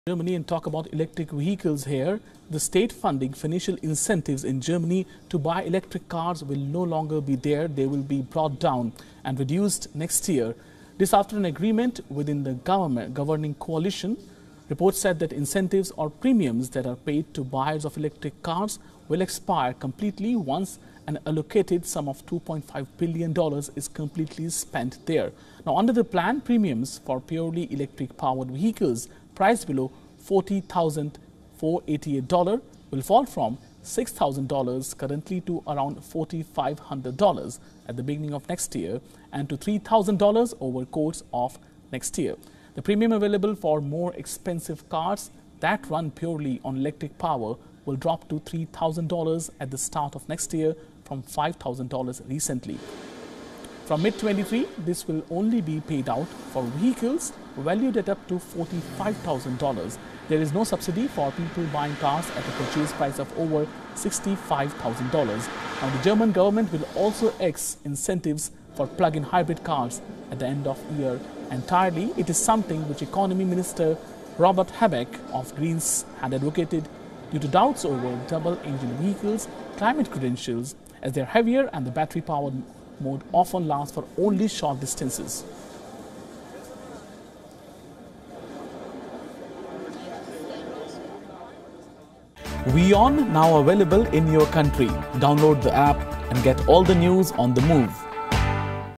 Germany to reduce electric car subsidies in 2023 | Automobiles | Business News | WION
Germany will reduce financial incentives to buy electric cars next year after an agreement within the governing coalition, as the vehicles' growing popularity makes government subsidies unnecessary, Germany's economy ministry said on Tuesday.